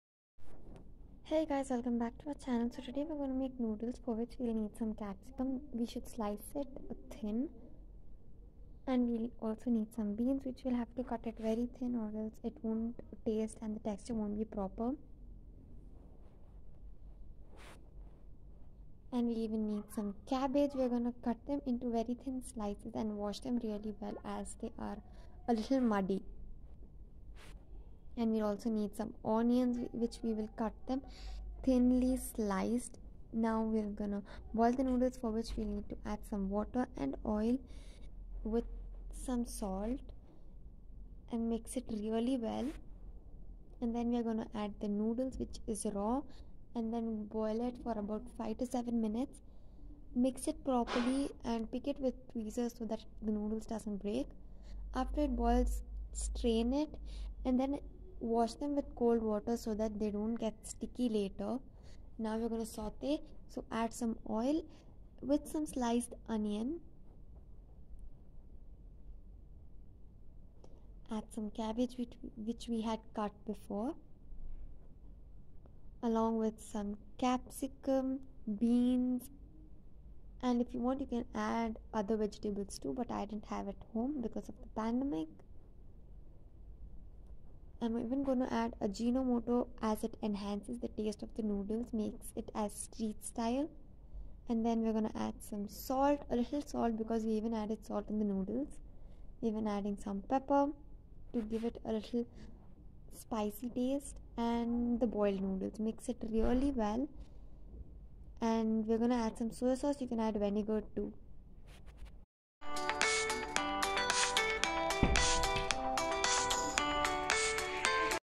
Street style noodles
Hey Guys⚡
Our new video is out now!
Lately i have been craving for street style Noodles like y'all

I have posted the whole video on our YouTube channel
Watch now!!!

So here is the yummy noodles i made hope you all like it!